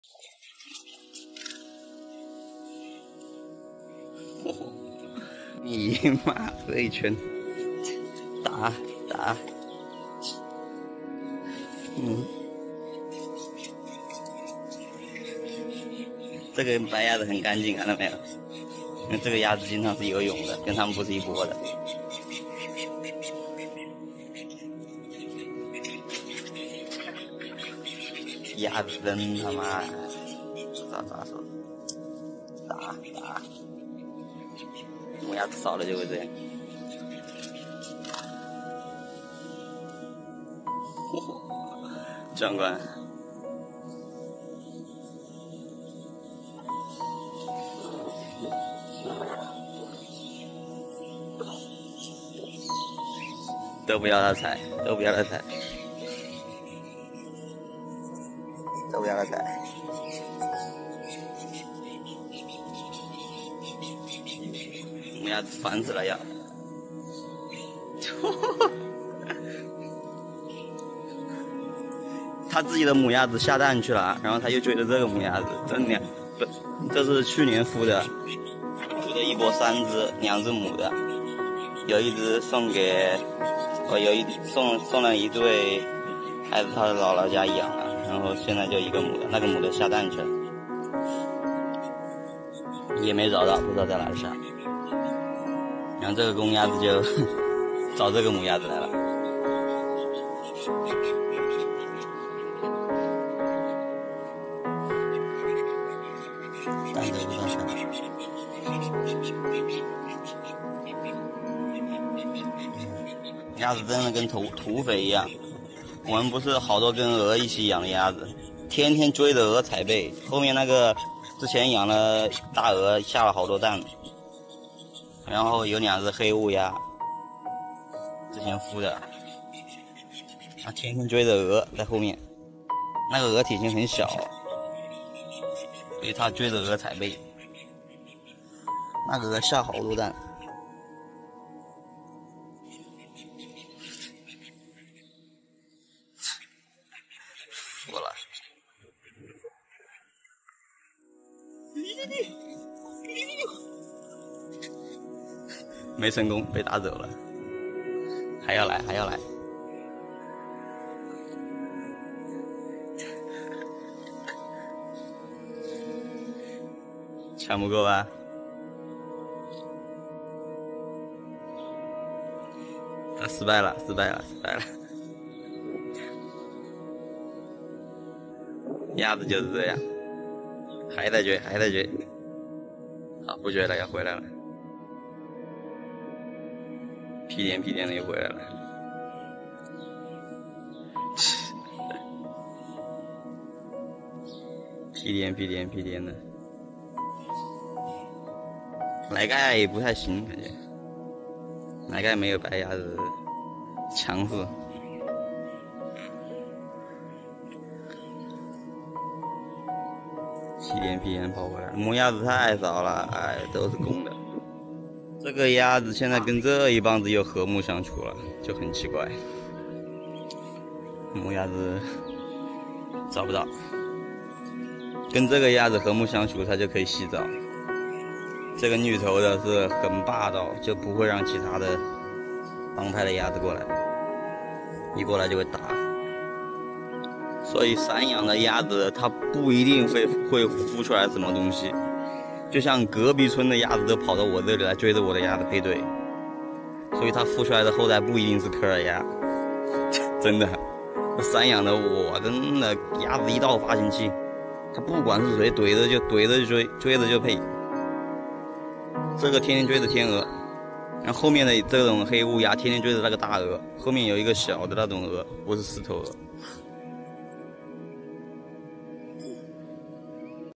為了母鴨子，三隻公柯爾鴨大打出手！
為了母鴨子，三隻公柯爾鴨大打出手！ | For the mother and duck, the three male Ko Duck ducks fought